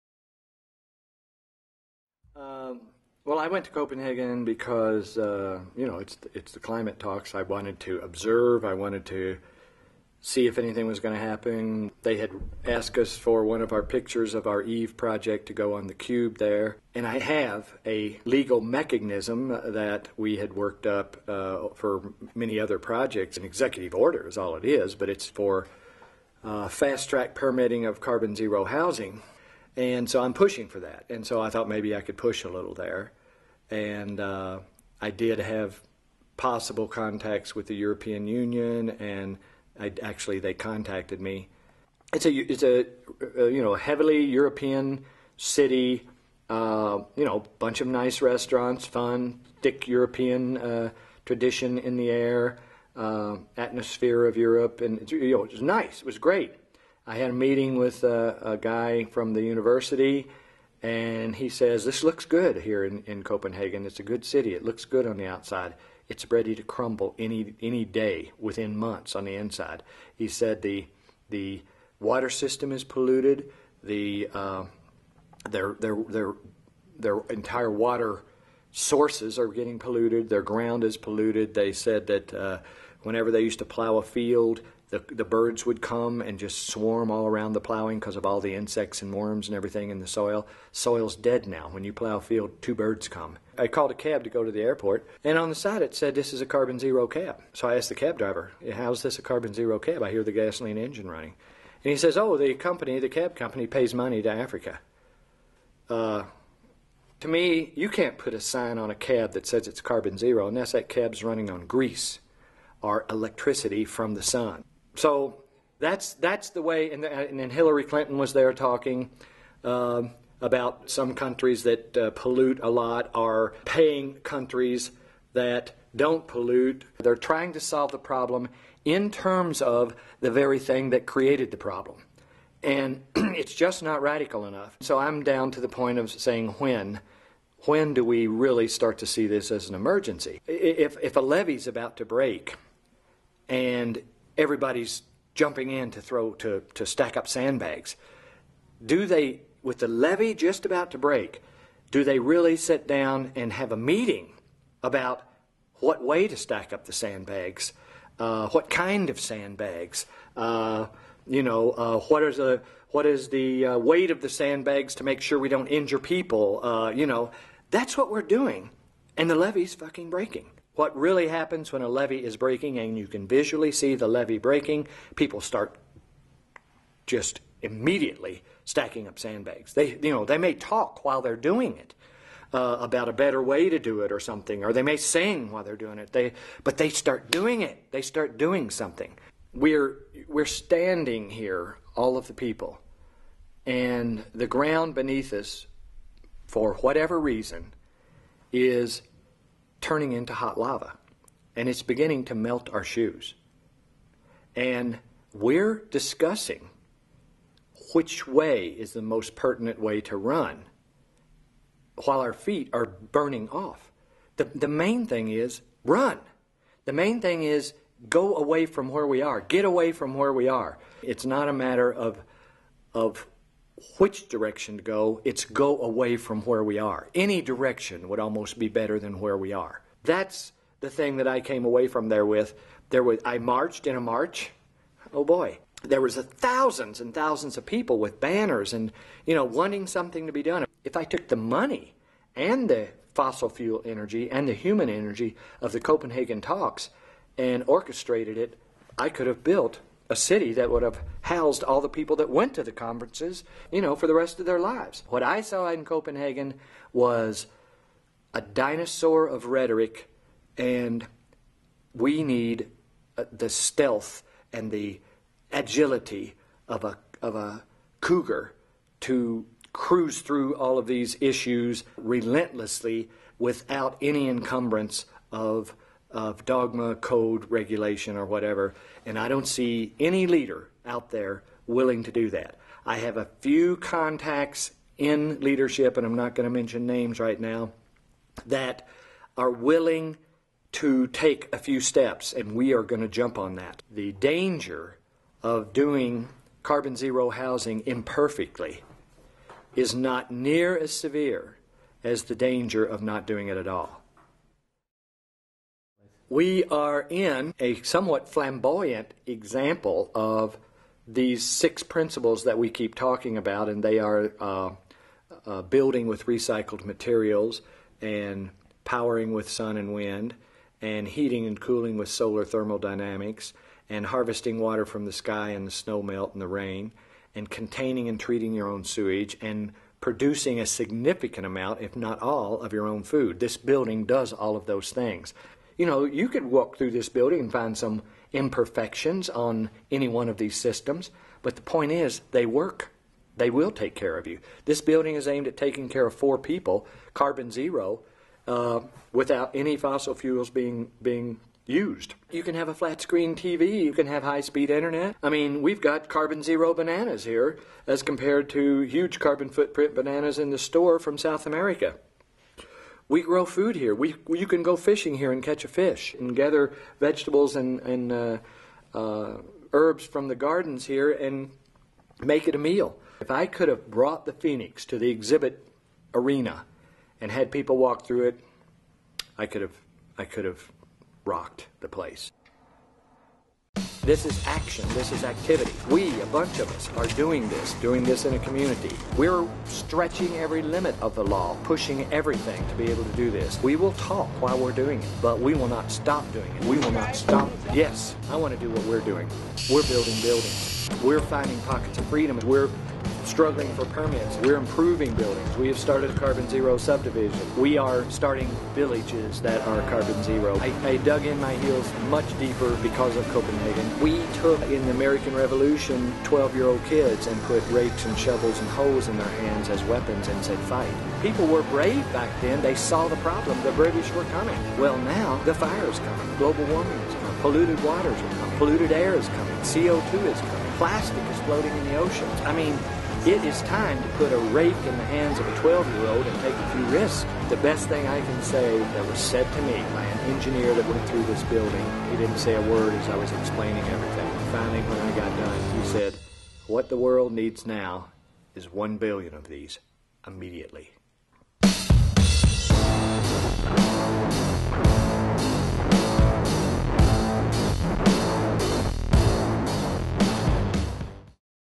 Earthship: Copenhagen
- Michael Reynolds of Earthship Biotectures shares his experiences at the Copenhagen Climate Conference.
An Earthship is a radically sustainable home made of recycled materials.